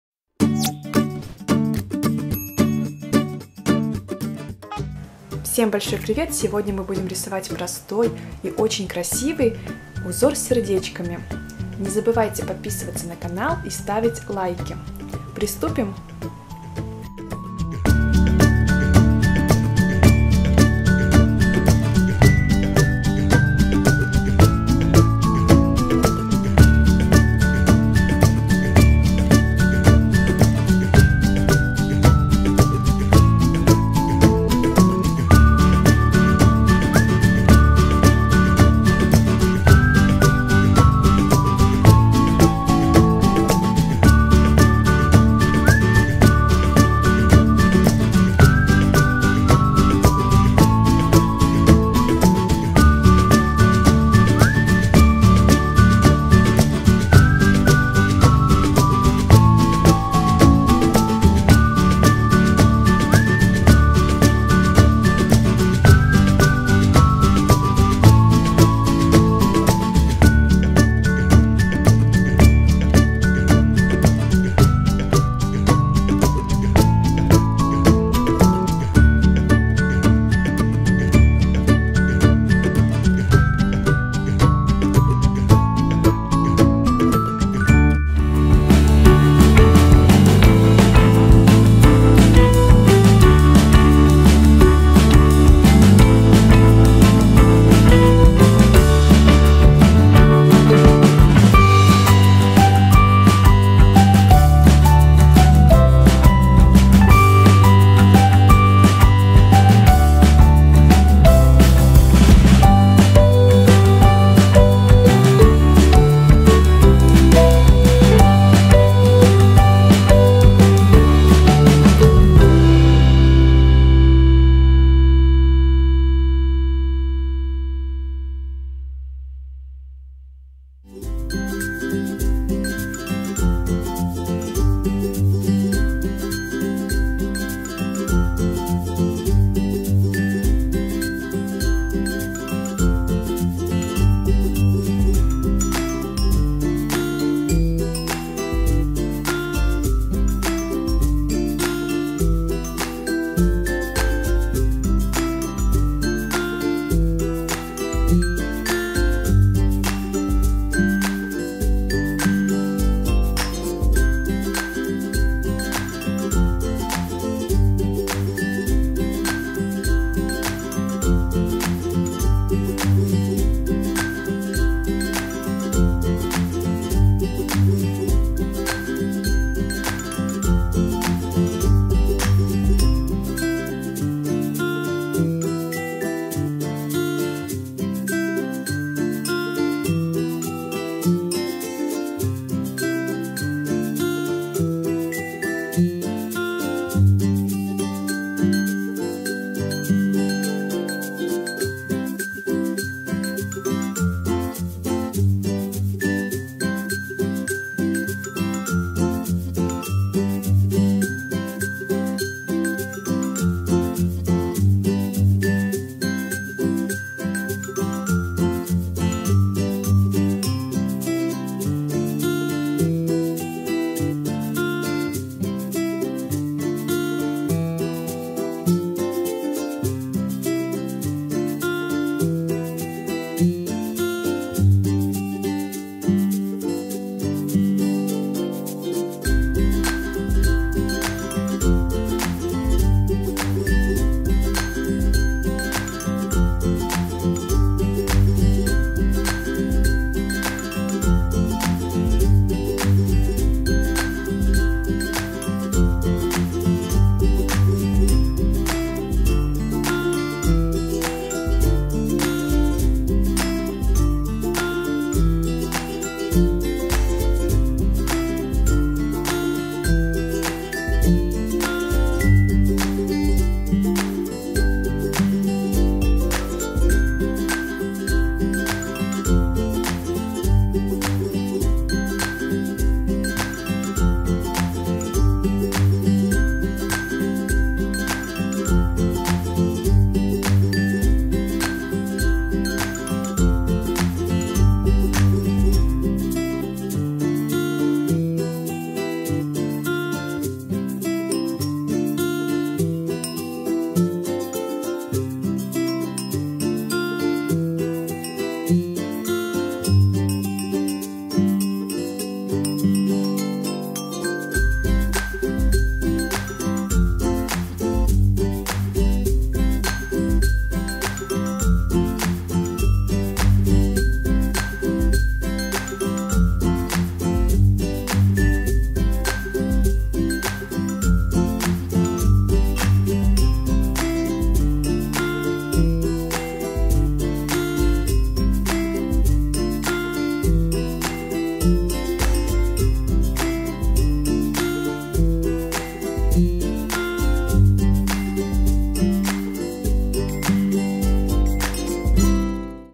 Как нарисовать простые рисунки / КРАСИВЫЙ УЗОР С СЕРДЕЧКАМИ / для срисовки
Всем привет! Сегодня мы рисуем простой и очень красивый узор с сердечками!
Рисовала фломастерами!